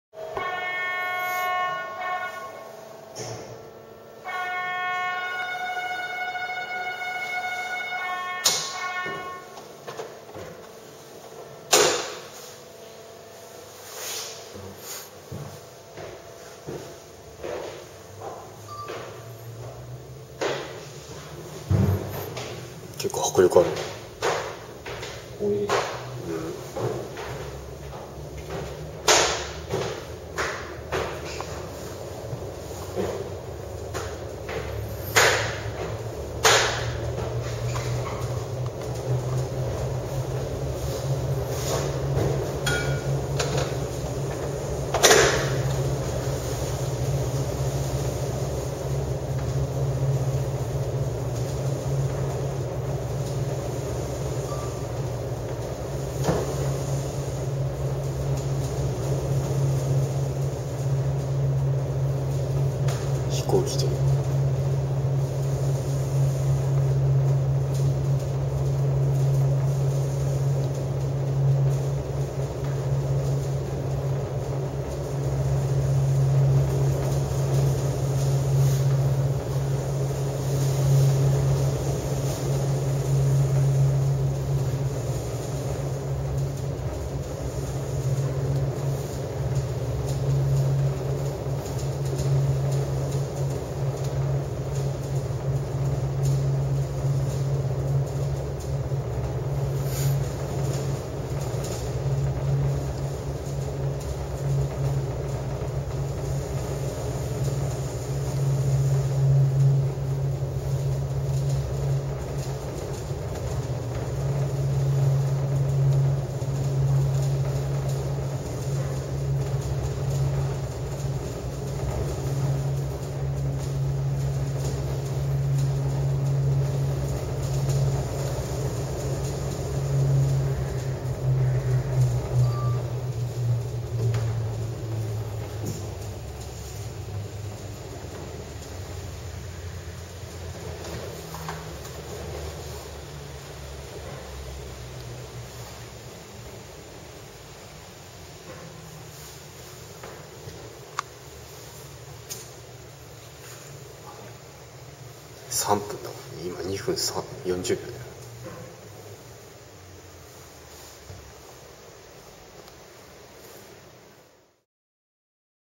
函館山ロープウェイ - Mt Hakodate Ropeway
函館山ロープウェイの頂上からの全行程です。
北海道新幹線で函館いらしたらいかがでしょうか？